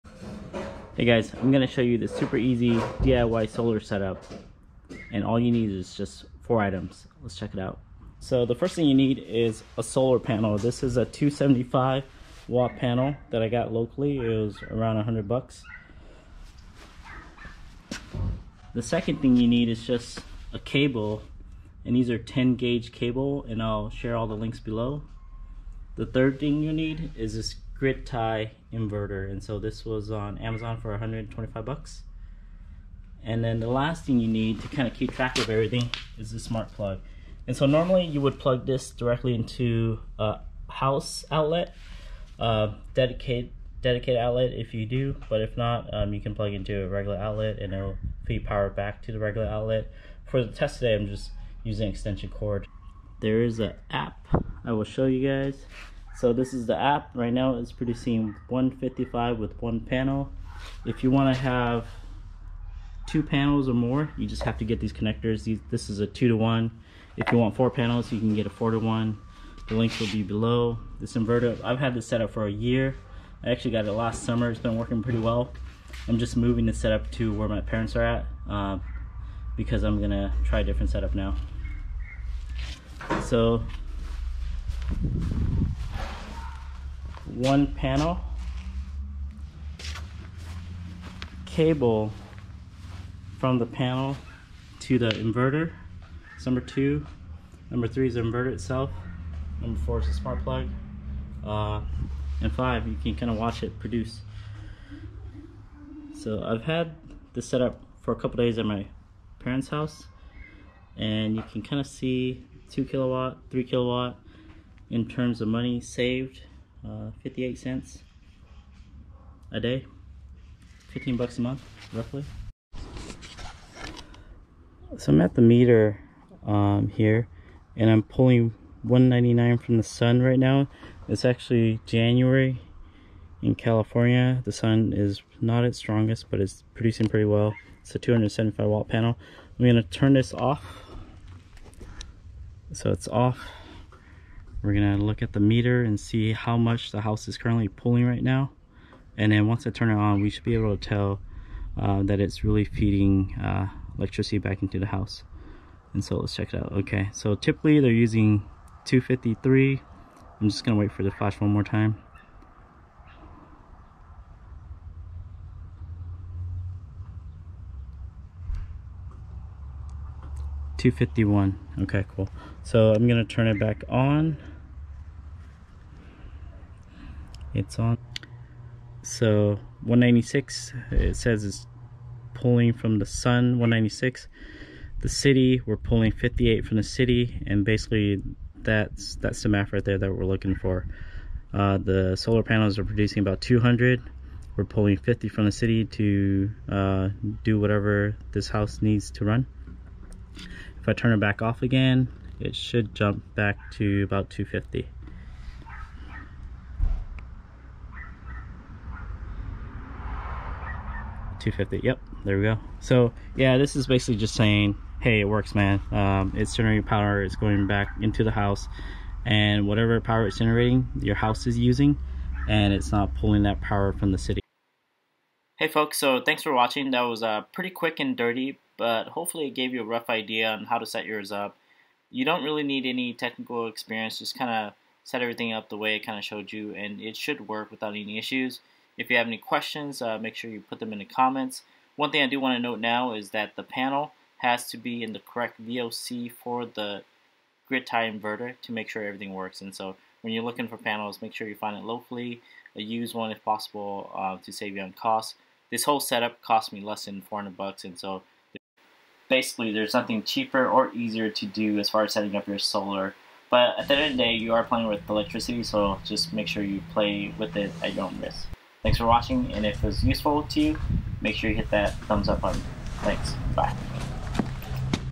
Cheap and Easy DIY Solar Panel Setup 1000w Amazon Grid Tie Inteveter
Hey Guys, this is the cheapest and easiest way I know how to setup a DIY Solar setup for less than $400 bucks. This is a grid-tie so it will help feed solar power back to your house when you have electricity running. This system does not work off grid or will not work for a backup situation when you have no power.  Here is the list to all of the items I purchased.

Y&H 1000W Grid Tie Inverter
10 awg cable
MC4 4 to 1 Connector
MC4 2 to 1 Connector
Etekcity Smart Plug

Panels were source locally because they are very heavy (45 pounds) and shipping cost is pretty crazy. Check craigslist or Facebook marketplace. VOC should be between 36V to 46V. If you are in Sacramento, I can get them for you for $120 per panel.